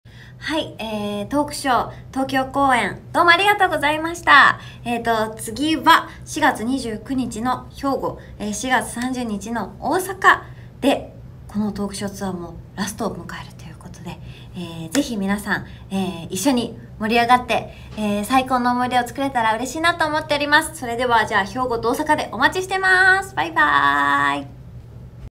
宇野実彩子さんよりコメント到着！【宇野実彩子 TALK SHOW 2023】
ソロデビュー5周年を記念し、自身初となるトークショーイベントを開催！
宇野実彩子 TALK SHOW 2023
🚢兵庫公演🚢
日時：2023/04/29(土・祝)
　　　〈1部〉開場 12:30 / 開演 13:00
　　　〈2部〉開場 16:00 / 開演 16:30
会場：あましんアルカイックホール・オクト

🐙大阪公演🐙
日時：2023/04/30(日)
　　　〈1部〉開場 12:30 / 開演 13:00
　　　〈2部〉開場 16:00 / 開演 16:30　SOLD OUT!!
会場：メルパルクホール

＼＼ 当日券情報 ／／
チケット料金：全席指定¥6,000
場所：開場時間より正面入口当日券売り場にて